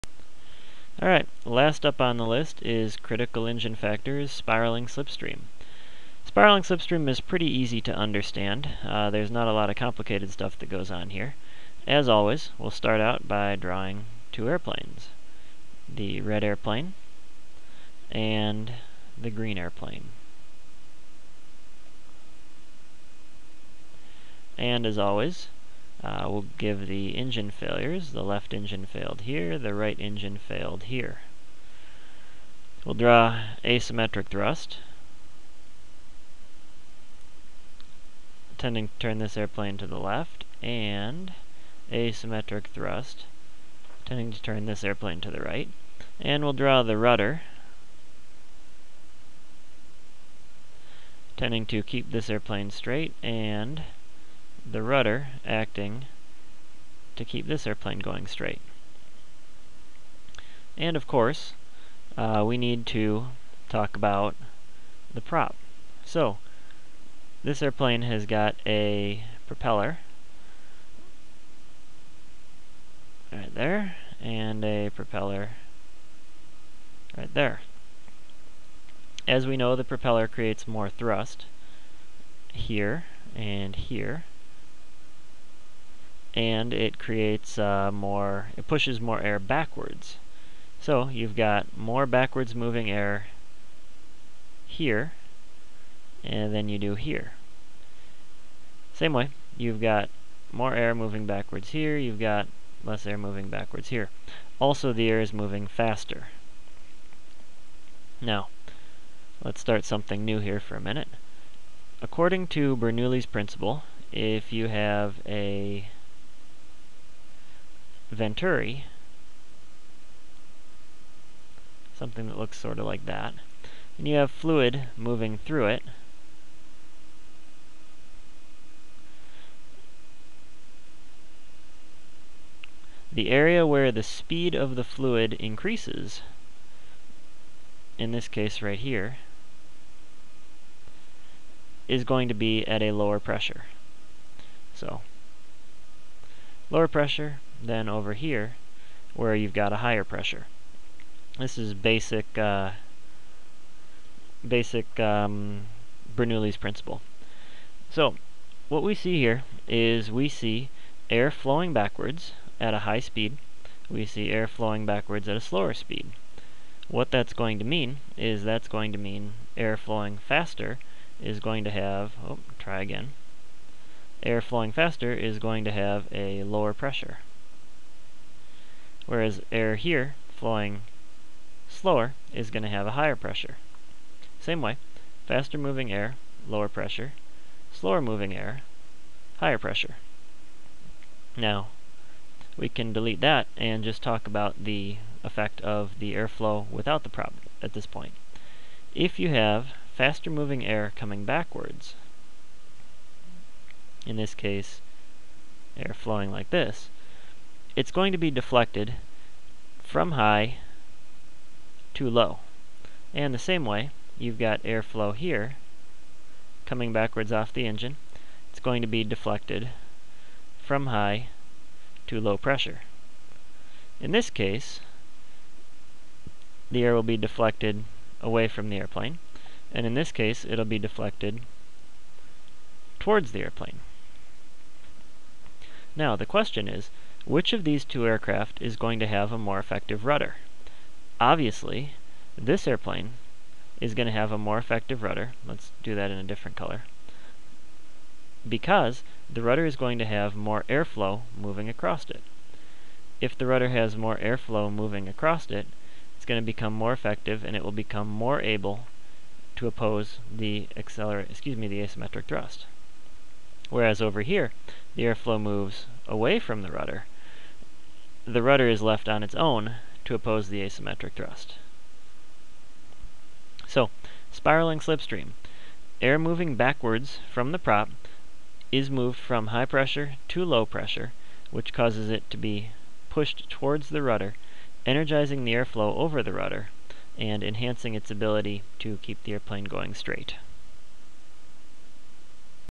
Critical Engine Factors: Spiraling Slipstream
The last in the critical engine series.  Remember that like all the other tendencies which ultimately result from P-Factor, spiraling slipstream only has an effect in a climb or descent.